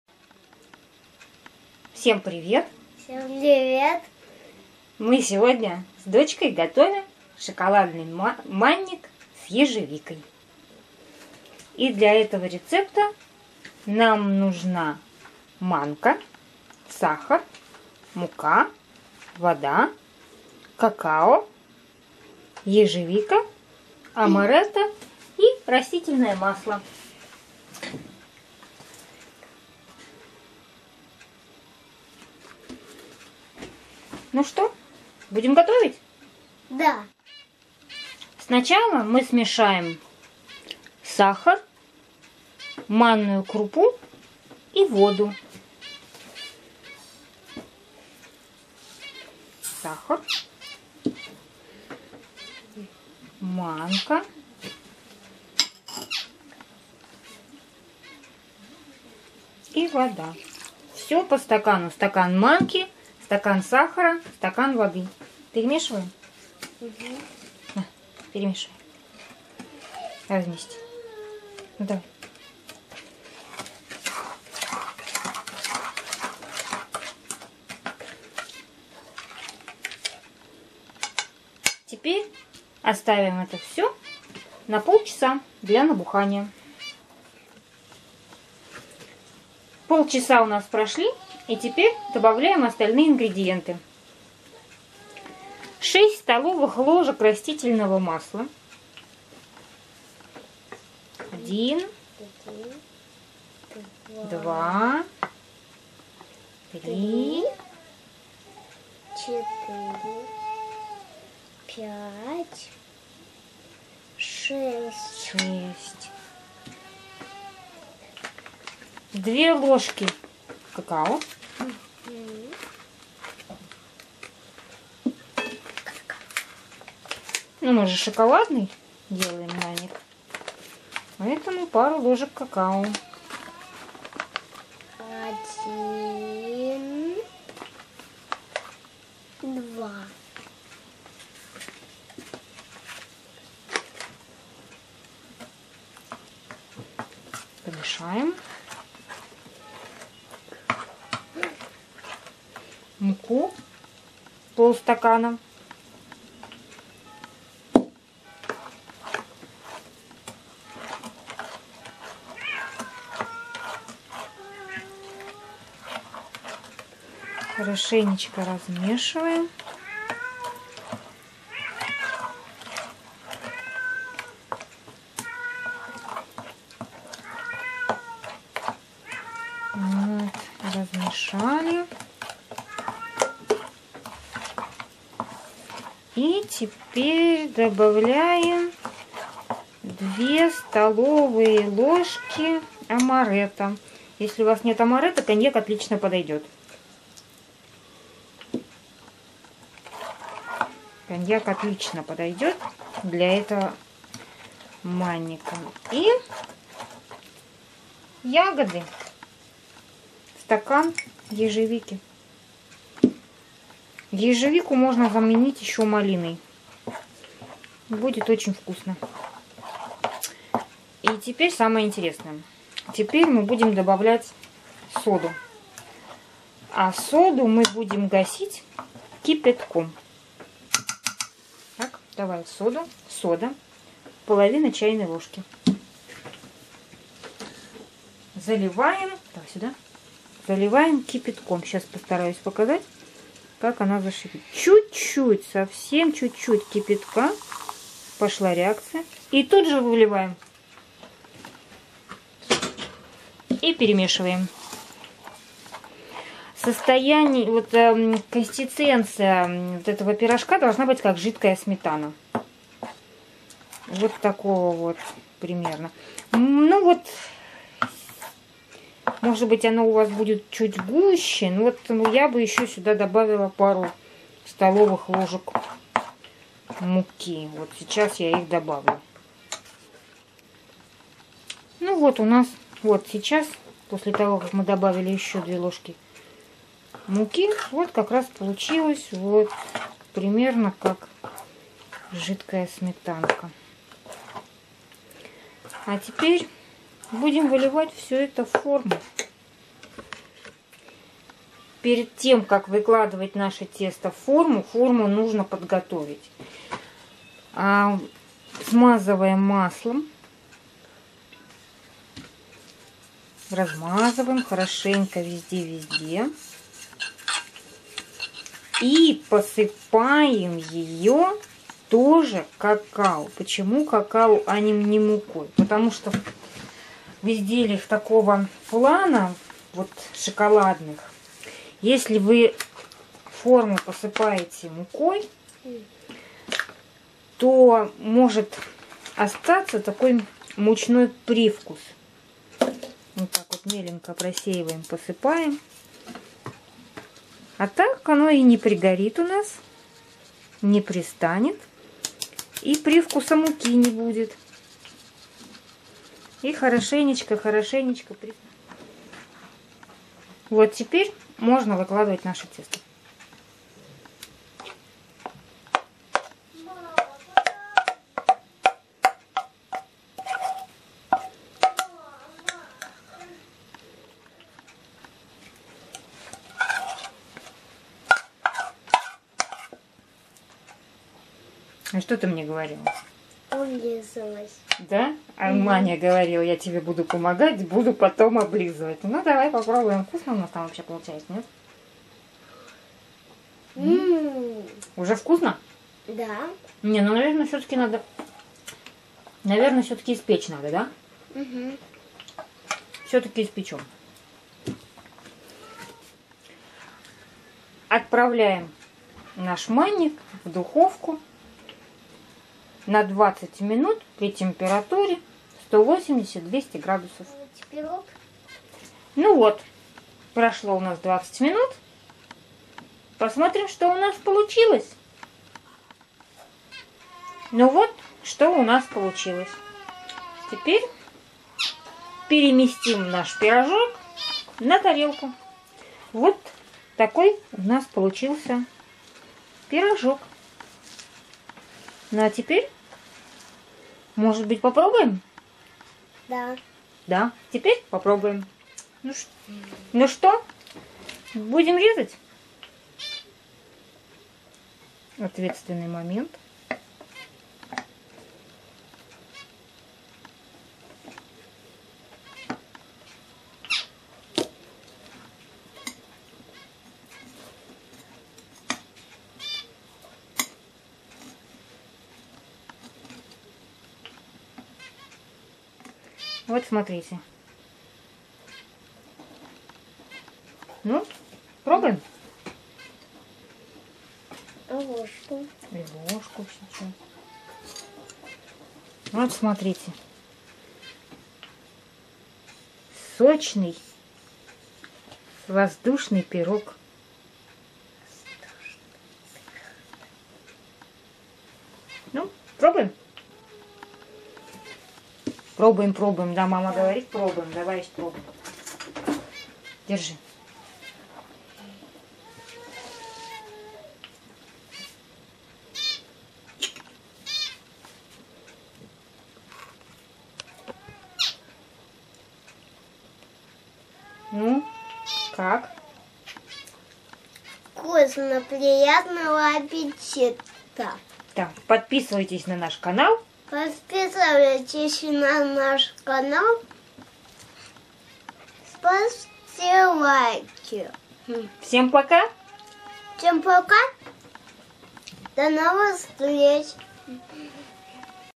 Шоколадный манник с ежевикой. Постный
Вкус этого манника приятно удивит вас и ваших близких. А гости ни за что не догадаются что это постное блюдо